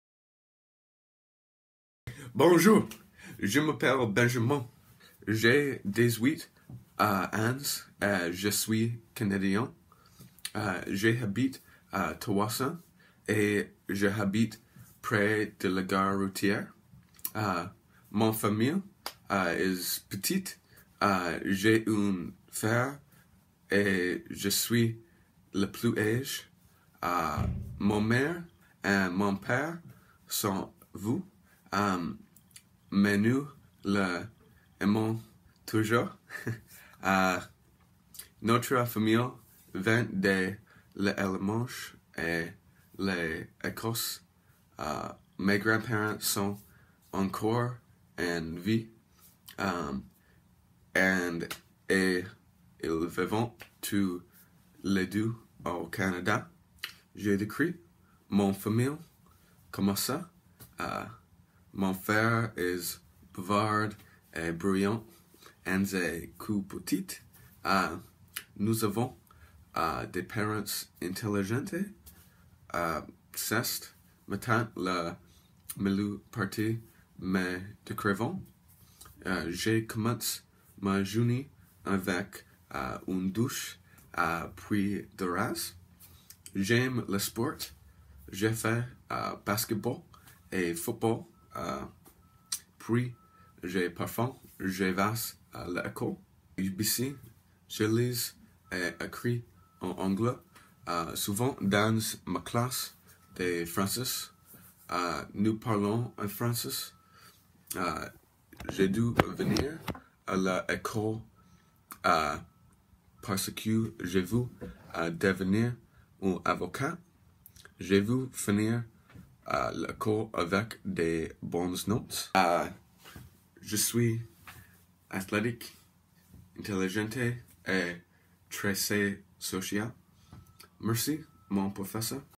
Présentation vidéo
Bonjour,  je m'appelle Benjamin

J’ai dix-huit ans et je suis Canadien

J’habite à tsawwassen et j’habite près de la gare routière

Mon famille est petit, j'ai un frère et je suis le plus âgé. Ma mère et mon père sont vieux, mais nous les aimons toujours haha.

Notre famille vient de l’Allemagne et de l’Écosse. Mes grands-parents sont encore en vie et ils vivent tous les deux au Canada.

Je décrirais ma famille comme ça …

Mon frère est bavard et bruyant ainsi que petit. Nous avons des parents intelligents.

C'est maintenant la meilleure partie, me décrivant

Je commence ma journée avec une douche puis je me rase.

J’aime le sport, je fais basket-ball et football puis parfois je vais à l'école

À UBC je lis et écris en anglais, souvent dans ma classe de français nous parlons en français

Je dois venir à l'école parce que je veux à devenir un avocat. Je veux finir l’école avec des bonnes notes.

Je suis athletique, intelligente et très sociale

Merci mon professeur